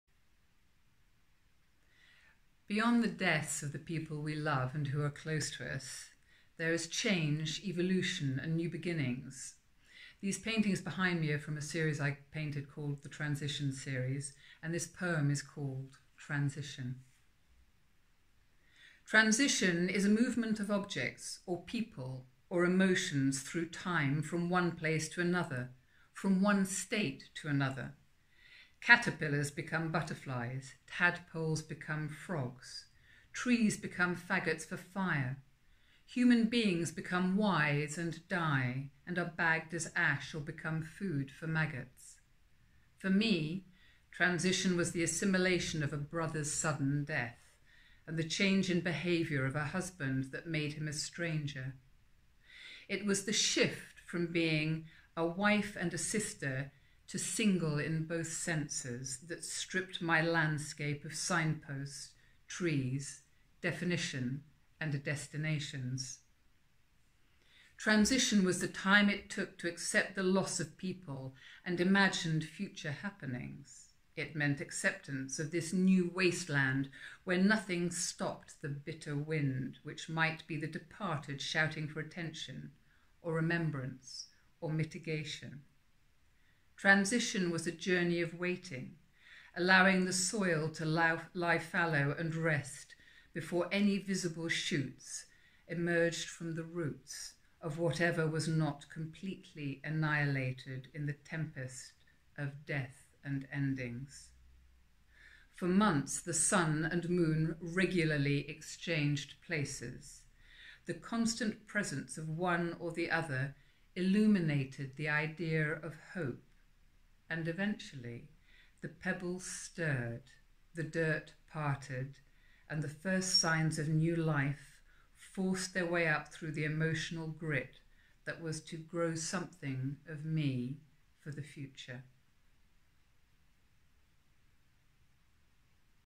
Transition - from Alternative Values by Frieda Hughes
What do you do when those you love die?  Re-negotiate life by means of a period of transition, through misery, unhappiness, repair and eventually, recovery.